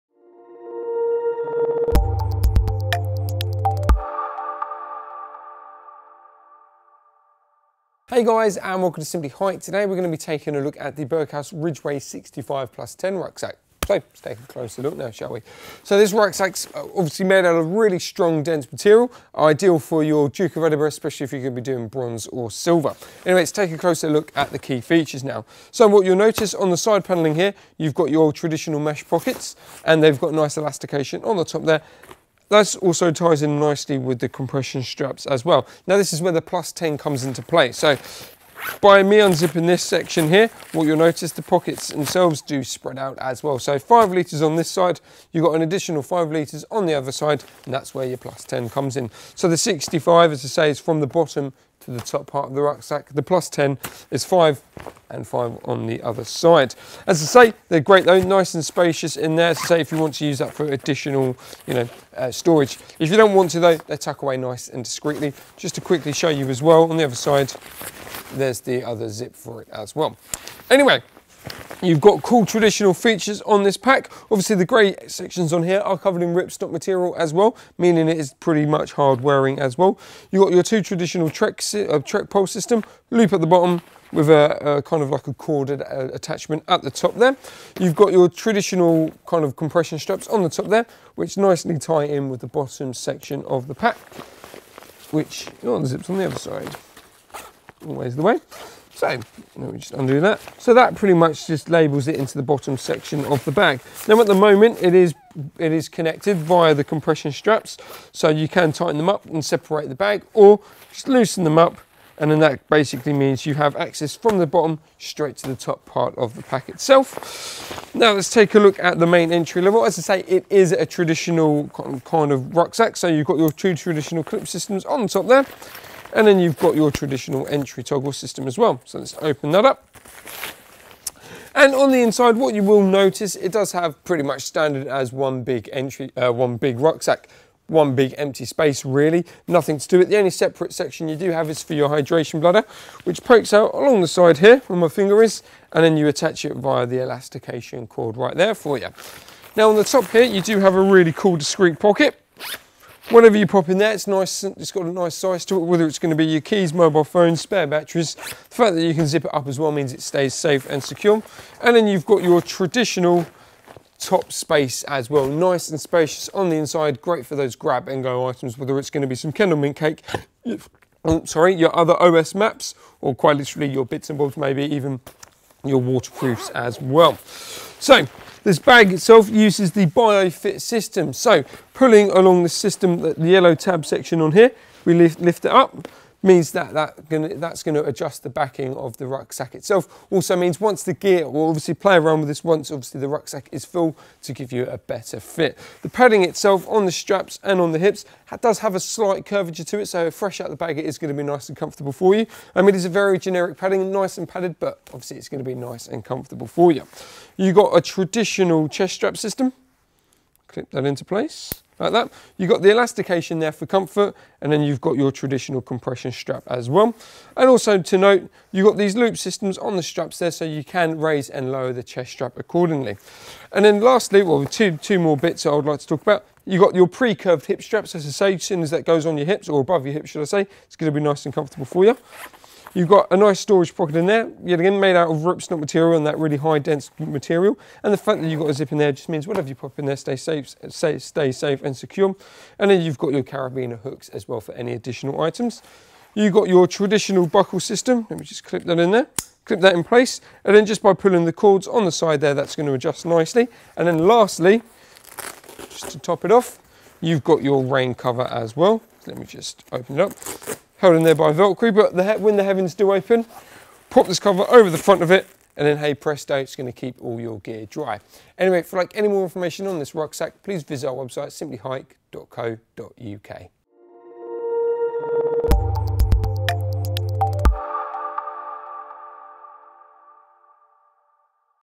Berghaus Ridgeway 65 Plus 10 Rucksack
The Berghaus Ridgeway 65 + 10 rucksack is a great, new pack with 'on the move' adjustability, great for those who like to really go into the wild. Packed full of features, the Ridgeway will carry plenty of gear while caring for your body as you push yourself to the limit.